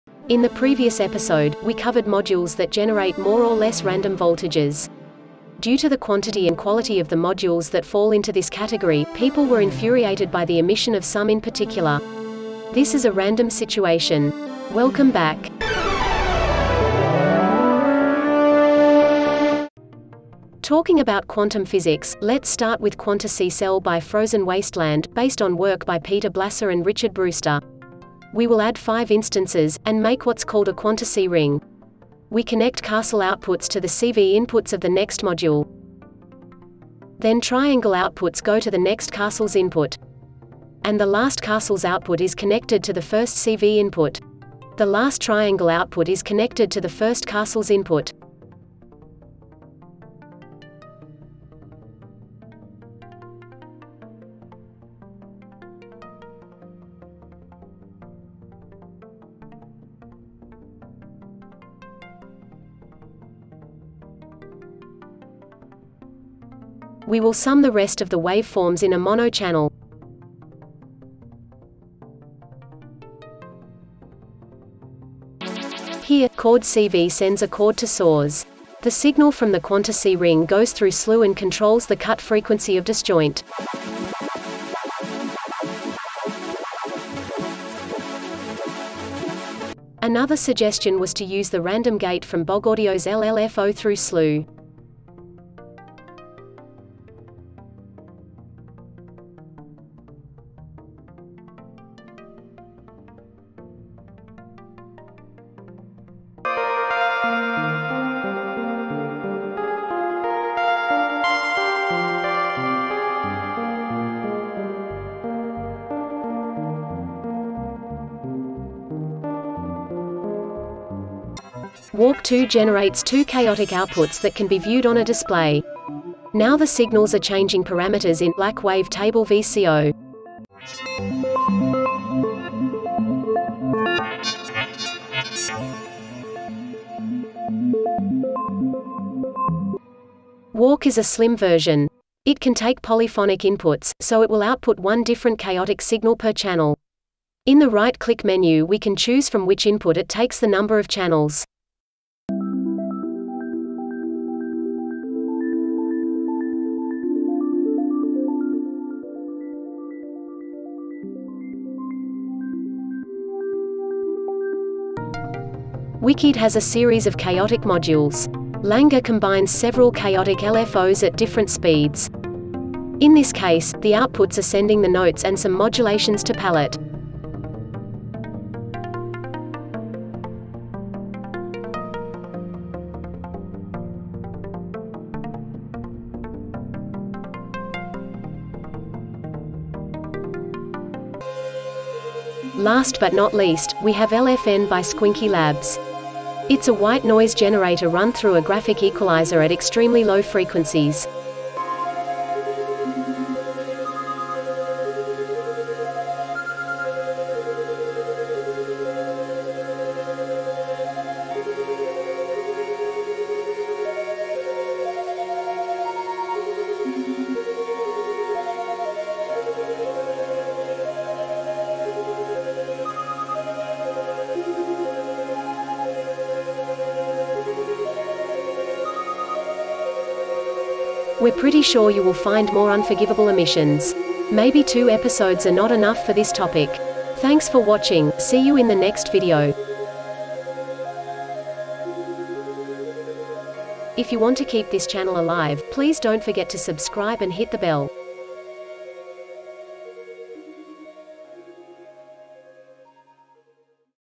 VCV Rack: More randomness to your patches.
Reviewed modules:

Frozen Wasteland :: Quantussy Cell
Bogaudio :: LLFO, WALK2, WALK
wiqid :: languor
Squinky Labs :: LFN

Voice: Google Cloud Text-to-Speech